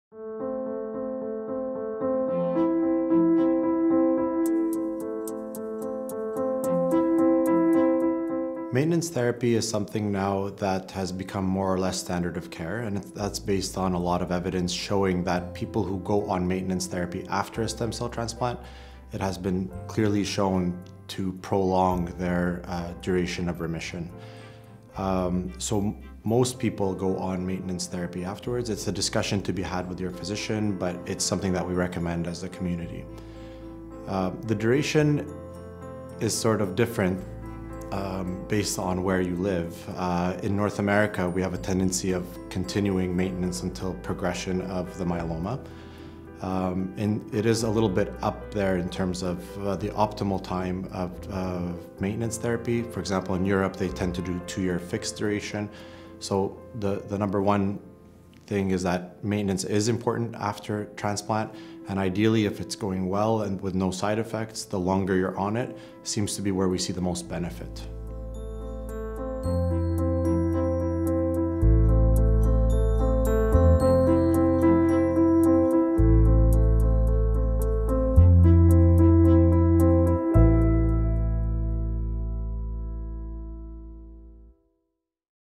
Myeloma Canada InfoVideo Series #3 - Maintenance Therapy
How important is maintenance therapy after receiving a stem cell transplant?

Featuring Dr Rayan Kaedbey, Hematologist from Montreal, Quebec